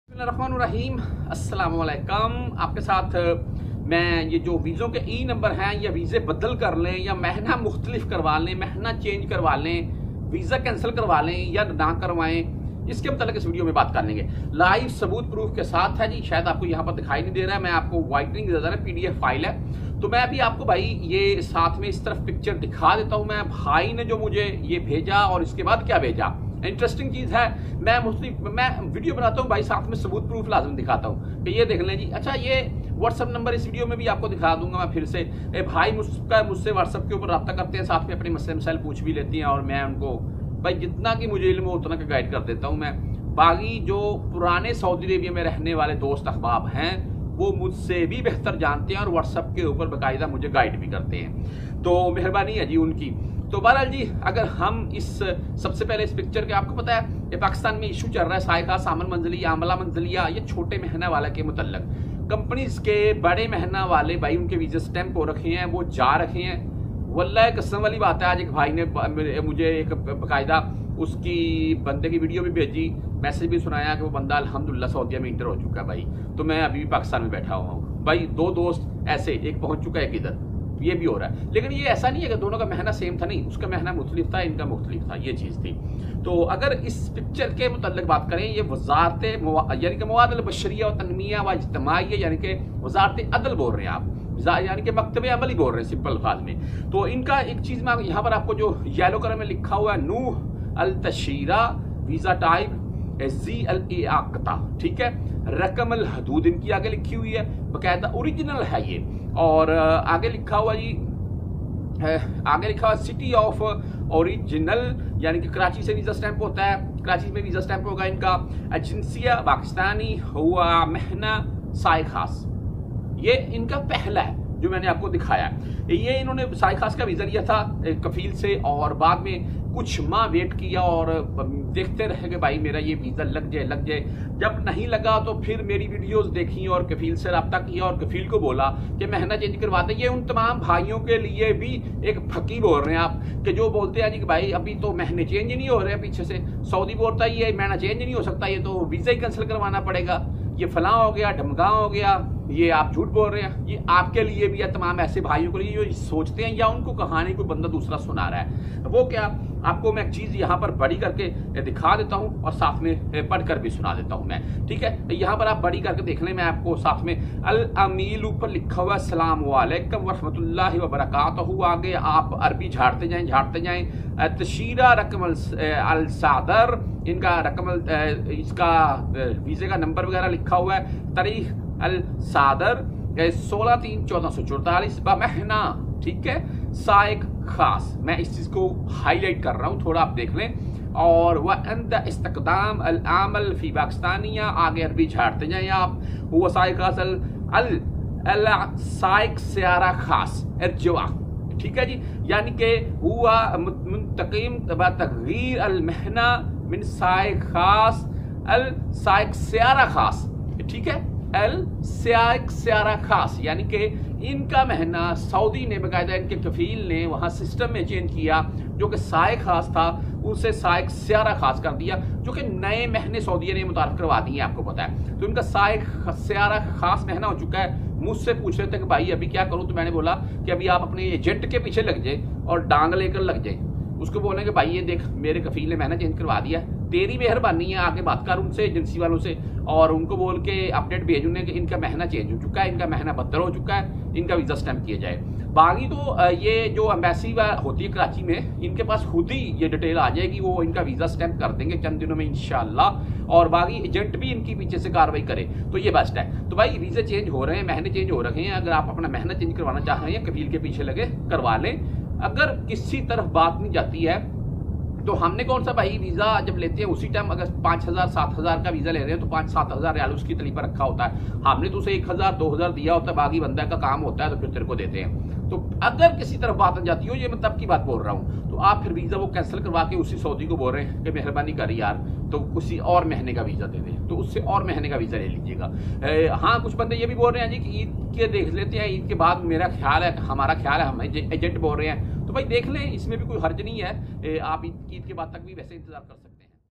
Saiq Khas and Amal Manzali Visa Change hona shuru | Saudi Arabia Visa E Number,
Saudi Arabia Latest Update News today in Urdu and Hindi, Saudi News in Urdu, Today News in Saudi Arabia, Saiq Khas and Amal Manzali Visa Change hona shuru | Saudi Arabia Visa E Number,